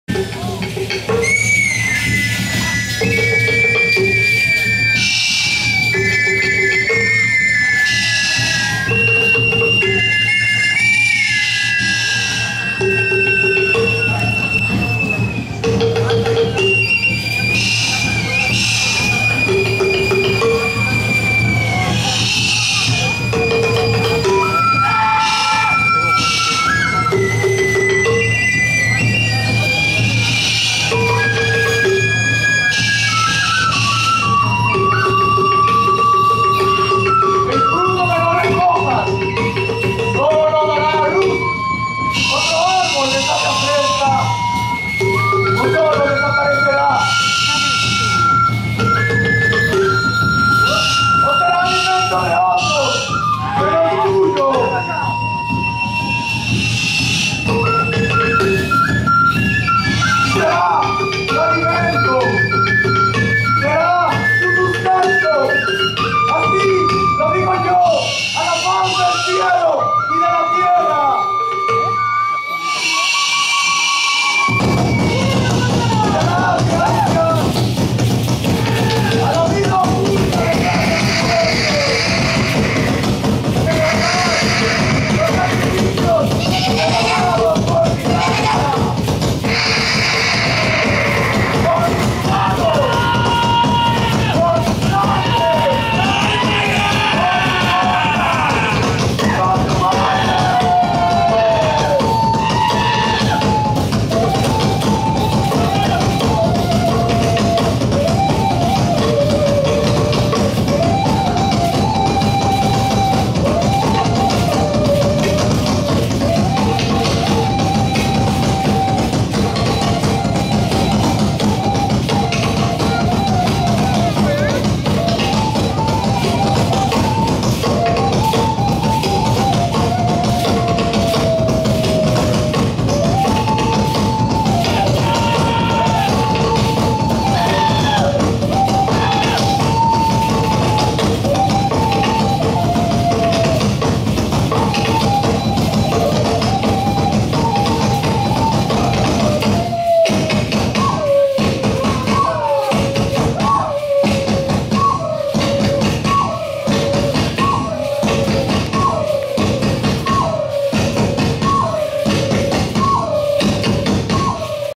473.MTS
Xcaret 11/2/2011 day of the dead, Pueblo Maya, Maya dance.